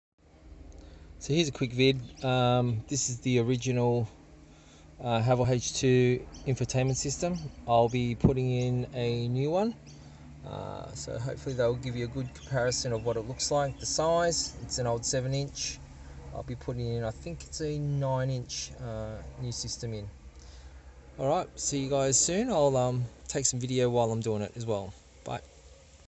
Original 2020 Haval H2 Infotainment
Just the start of removing the Haval H2 infotainment system. First video, more to come.
Let me know if there is anything specific you would like to know